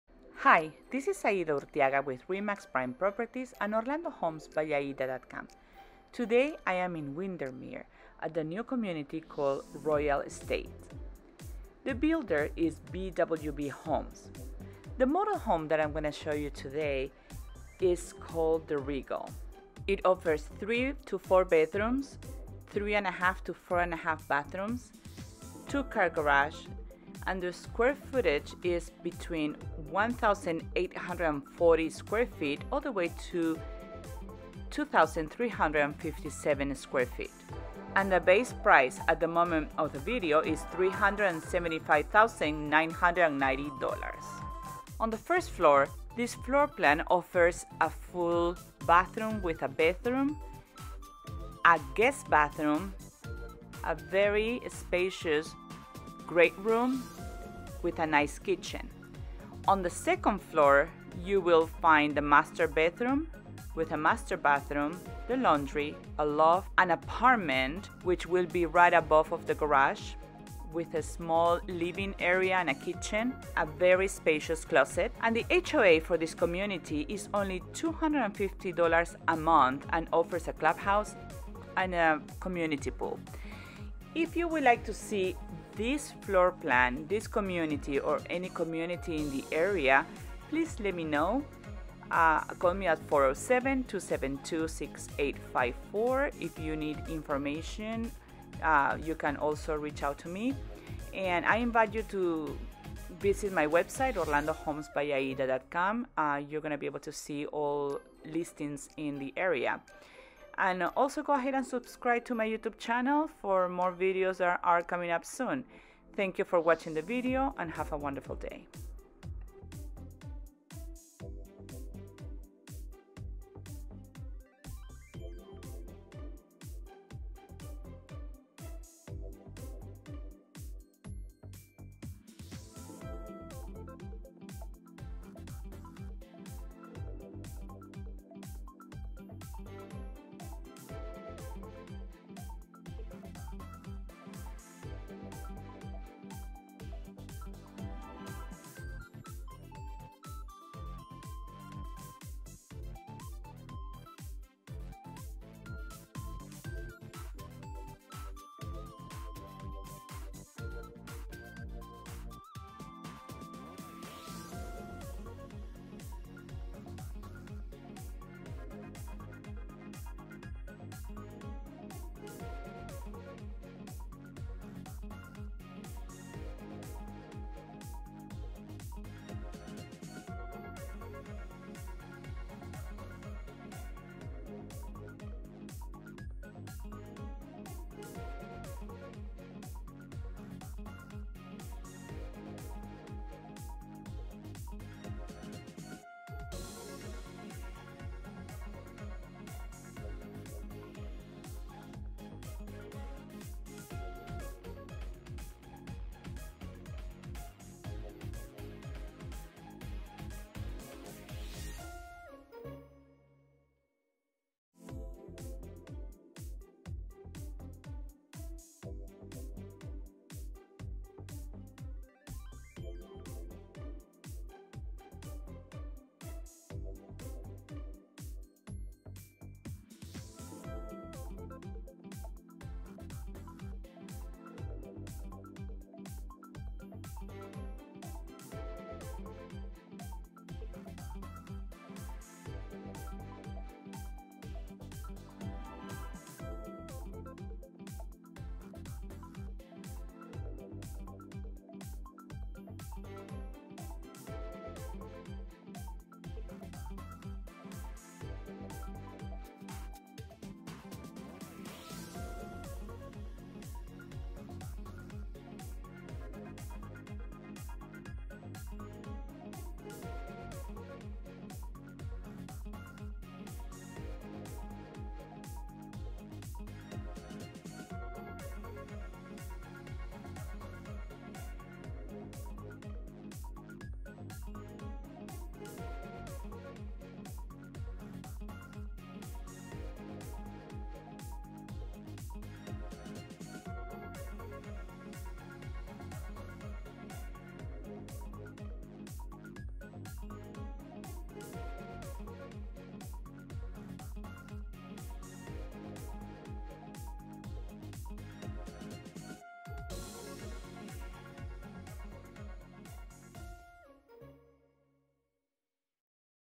Regal Model Home | Royal Estate in Windermere | FULL APARTMENT on the second floor
Regal Townhouse in Royal Estates, Windermere with an apartment on the second floor.

3 - 4 Bd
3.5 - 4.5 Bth
2 Car Garage
1,840 - 2,357 Sq Ft
$375,990 Base price at the moment of the view.

Aida Urteaga
Realtor
RE/MAX Prime Properties